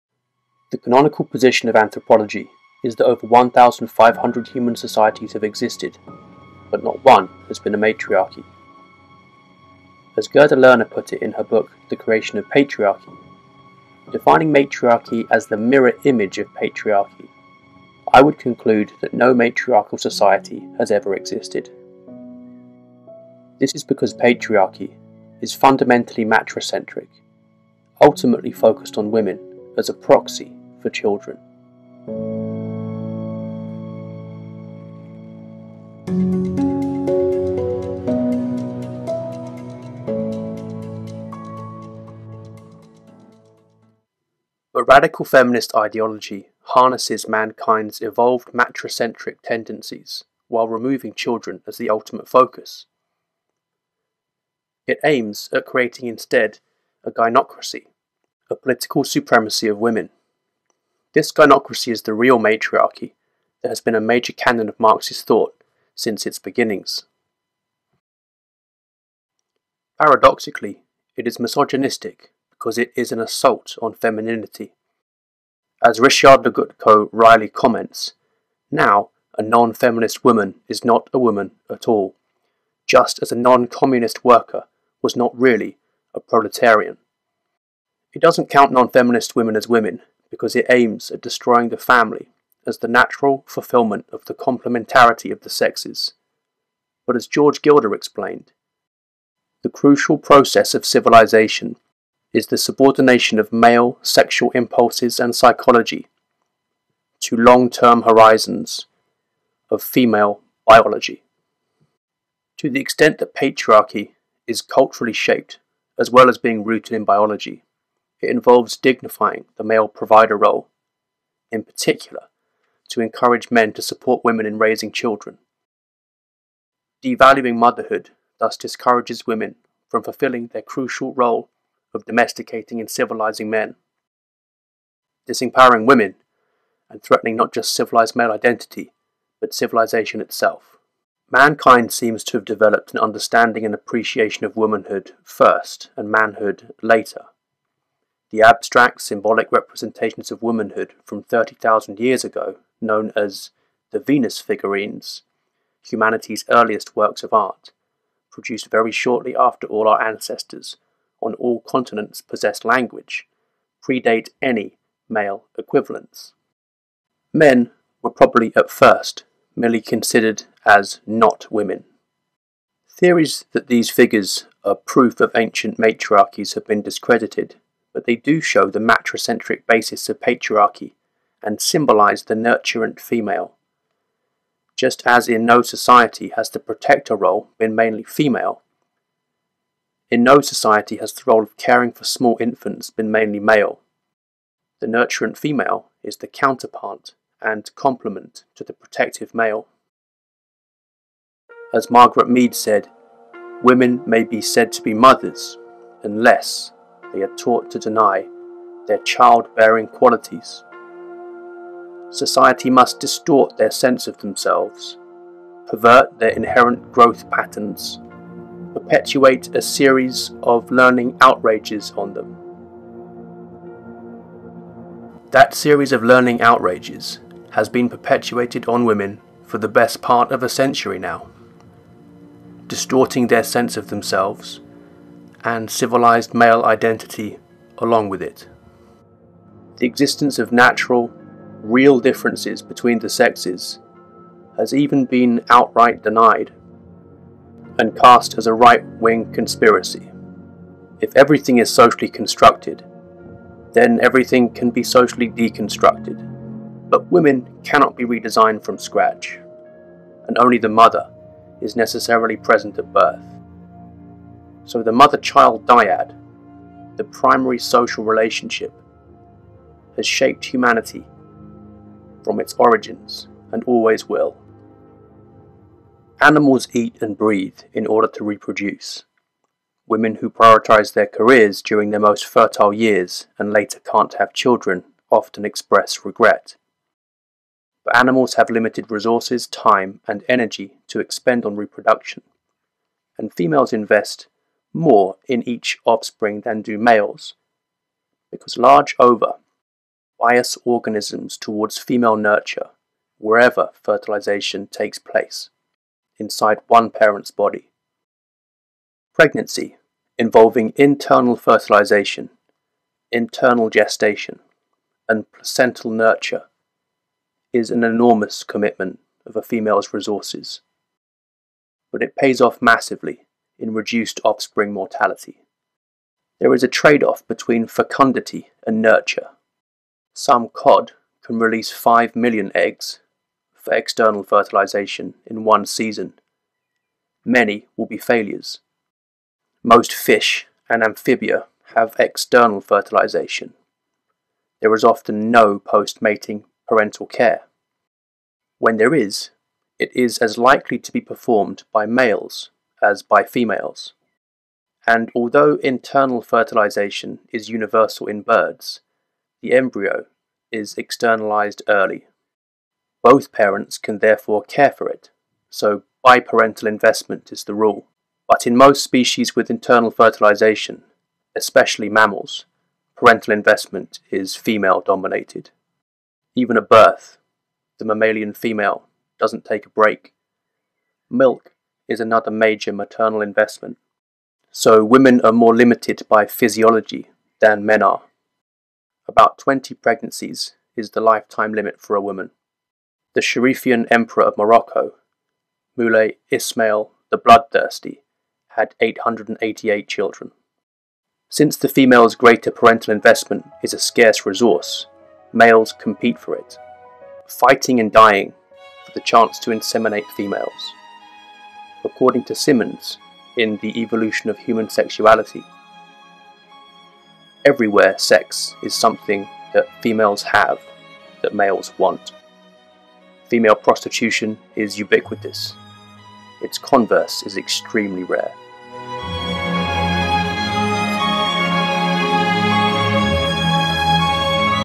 Womanhood and Motherhood
Why womanhood means the potential for motherhood. Part 1 of The Matriarchy Mystery.